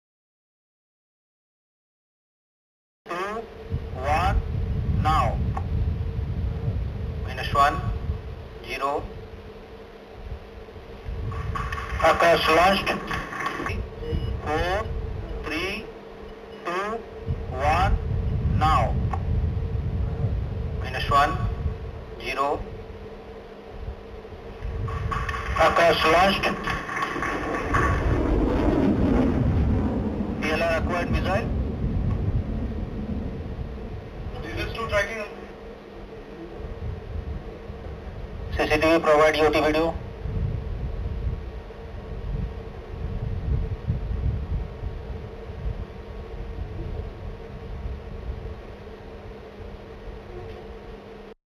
28 May, 2019 - India successfully test fires surface-to-air missile
28 May, 2019 (ANI) - India successfully test fires surface-to-air missile